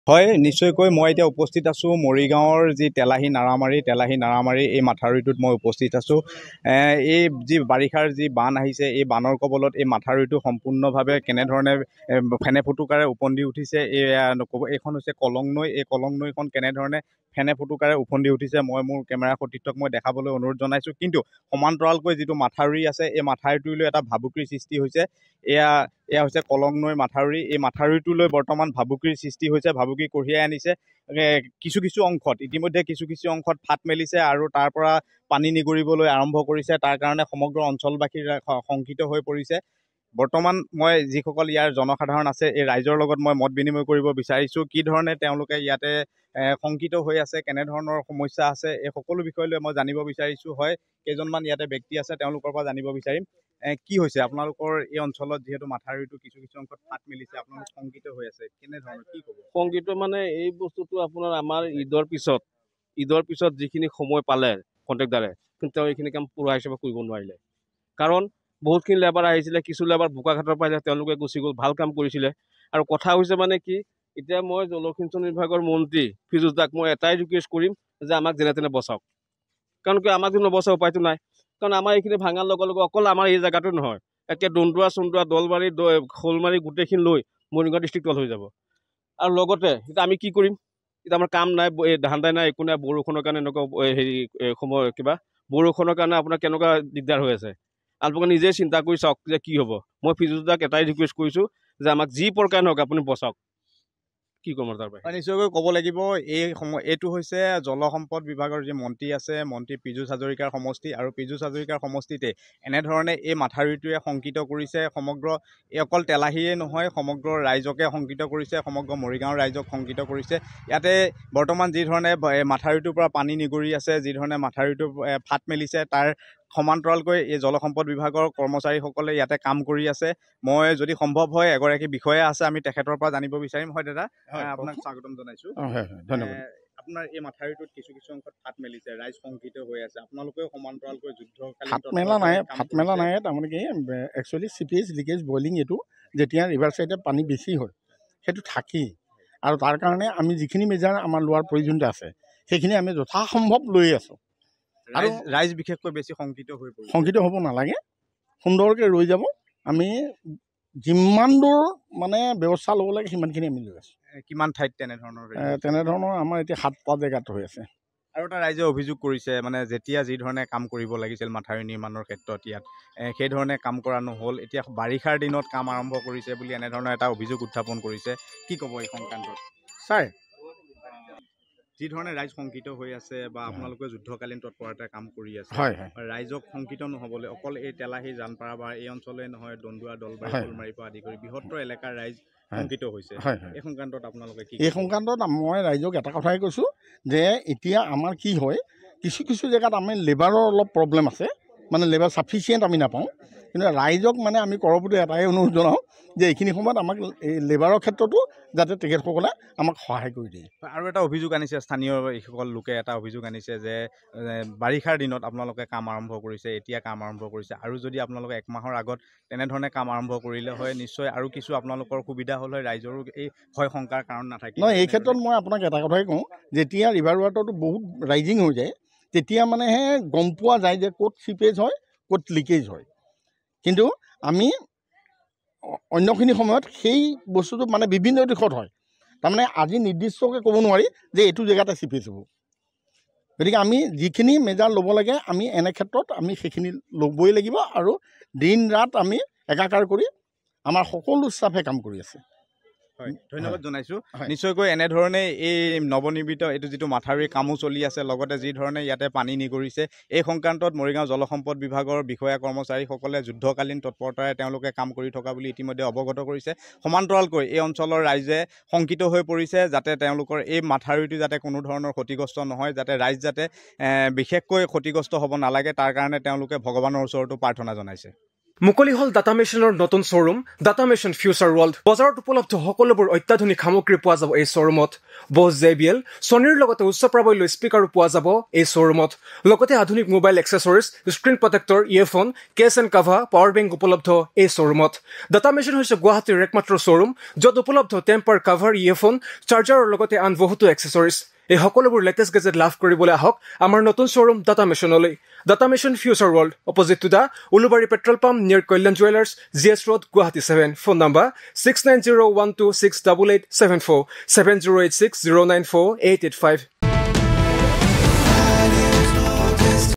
মন্ত্ৰী পীযুষ হাজৰিকাৰ গৃহ সমষ্টিত মথাউৰিয়ে আতংকৰ সৃষ্টি কৰিছে
My News NE:  মন্ত্ৰী পীযুষ হাজৰিকাৰ গৃহ সমষ্টিত  মথাউৰিয়ে আতংকৰ সৃষ্টি কৰিছে
কলং নৈৰ পাৰত থকা তেলাহী নাৰমাৰীৰ মথাউৰি খহায় নিয়াৰ আশংকা